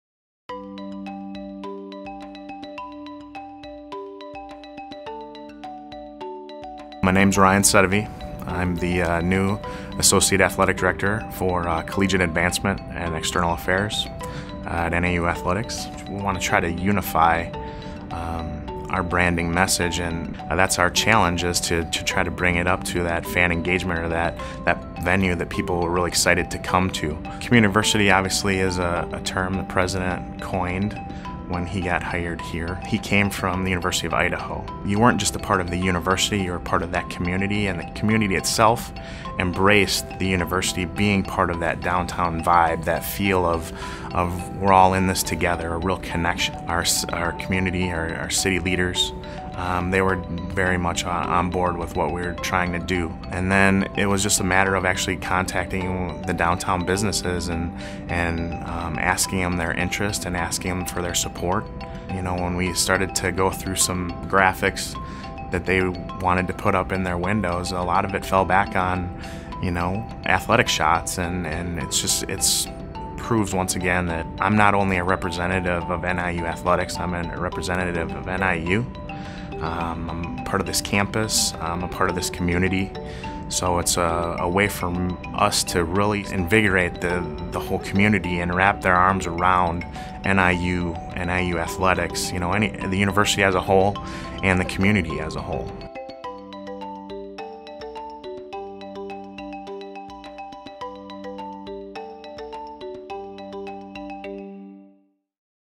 NIU & Communiversity HD
Northern Illinois University tells the story of Communiversity in the city of Dekalb, Illinois.  Kelmscott Communications applauds the efforts of NIU and the city of Dekalb.